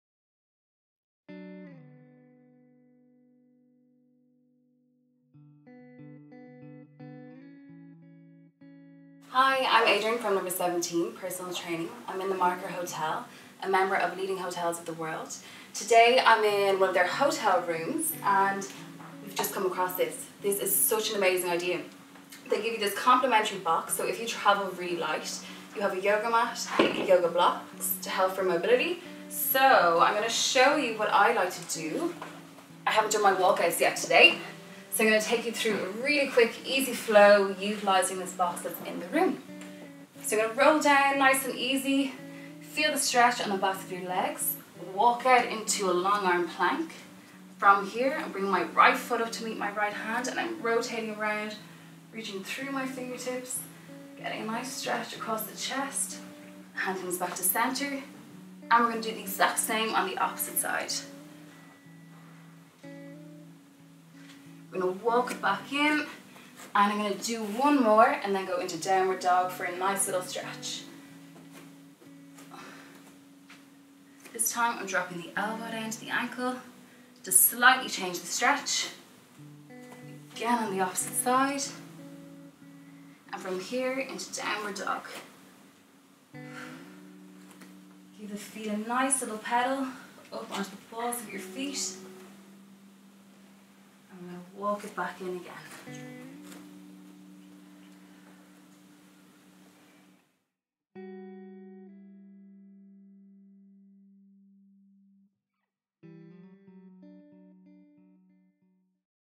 Hotel Bedroom Flow at The Marker Hotel
Adrienne Murphy, Personal Trainer with No.17 Personal Training at The Marker Hotel, takes you through an easy hotel bedroom flow using just a yoga mat!

Great for:
- After a long haul flight
- Stretching after sitting at a desk for long hours
- Include after any workout to improve mobility and stretch it out